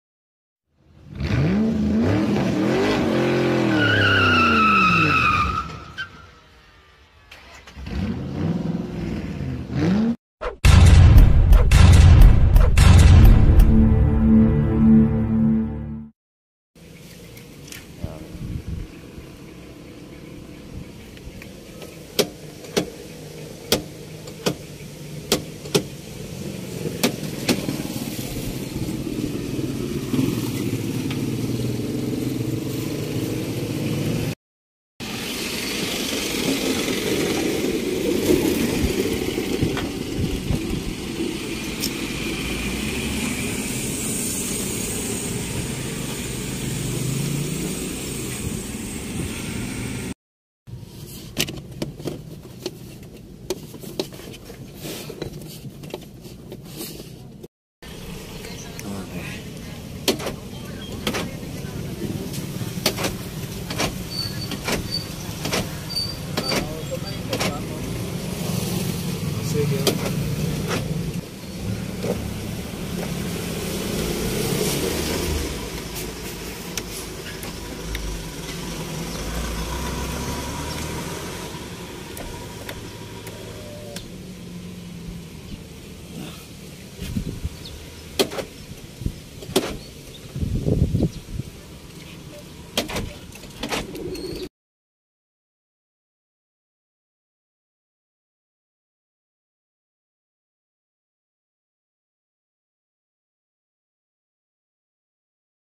Mitsubishi Adventure 2017 Power Door Locks not Working | Wires Bitten by Mice
Watch in the video Mitsubishi Adventure 2017 Power Door Locks not Working | Wires Bitten by Mice. the customer said his central doesn't work properly and while the car is running the central locks intermittently work then stop. Upon check up I saw the wires cut and seem to be bitten by Mice. Watch to learn more.